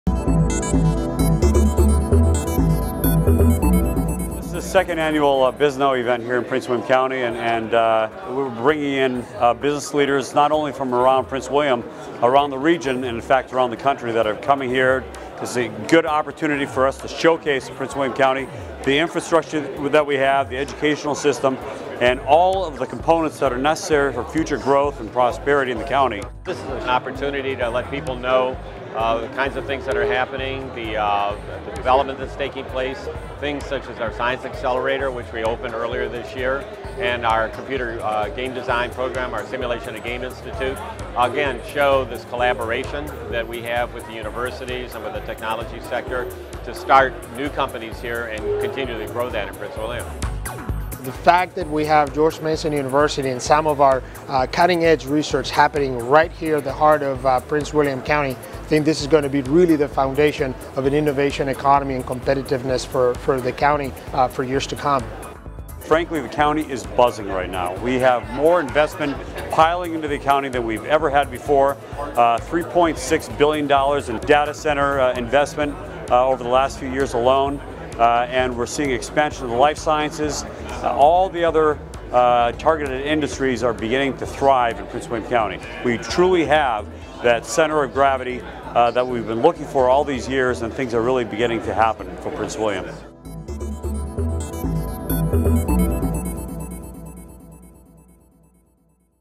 The Buzz: BISNOW 2014
The Prince William County Department of Economic Development hosted its second-annual Bisnow event at the Hylton Performing Arts Center this morning to showcase the benefits of doing business in Prince William County.